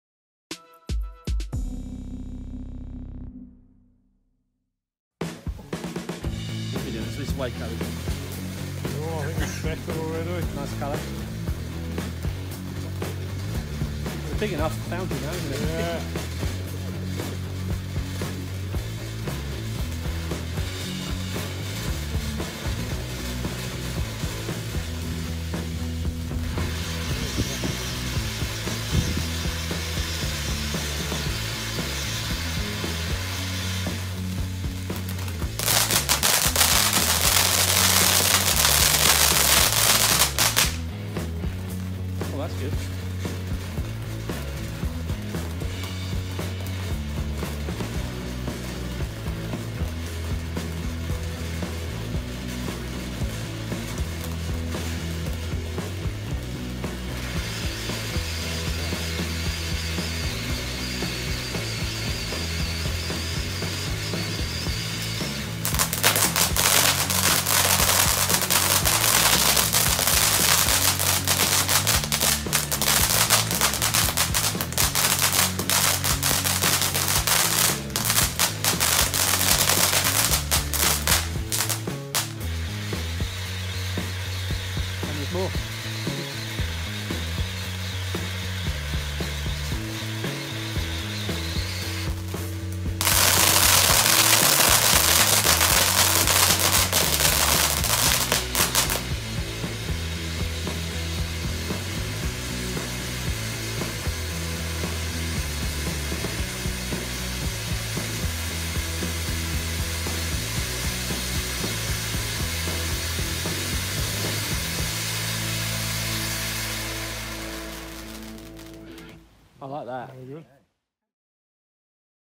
Firing Ocean Of Lights by Weco Fireworks.
In this video I test fire my first item from the German brand Weco Fireworks. This 'Ocean Of Lights' fountain battery had an outstanding duration and the effects were both mixed and varied with numerous transformations. NEC count 133g.